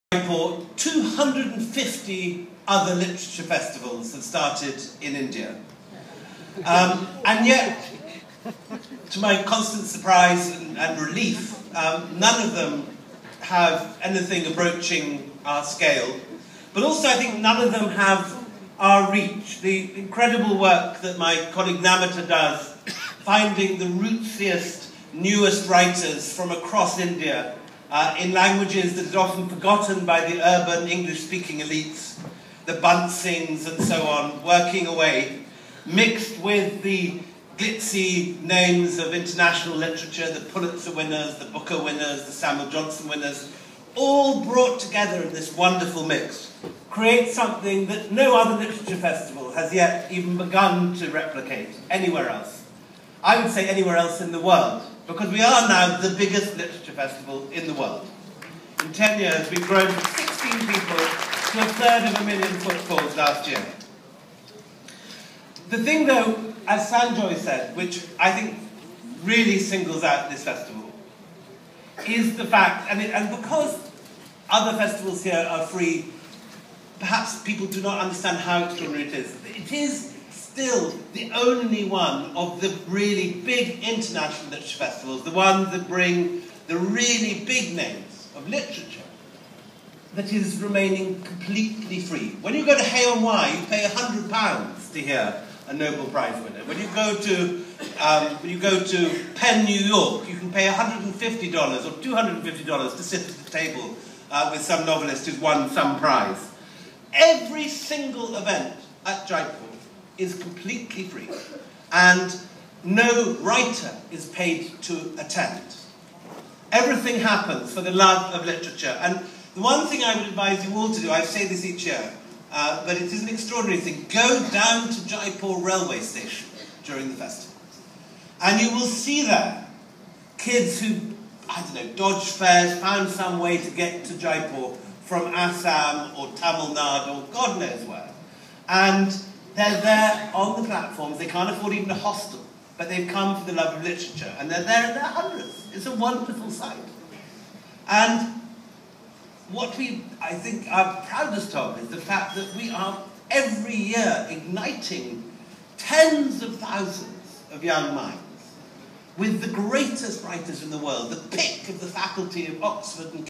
William Dalrymple speaking at the curtain raiser of 10 years of Jaipur Literary Fest
JLF 2017 to be held at Jaipur from 19 to 23 January celebrates the 10th year of the world's largest literature festival. Festival co-founder and director historian William Dalyrymple speaks at the curtain raiser event at Taj Mahal Hotel New Delhi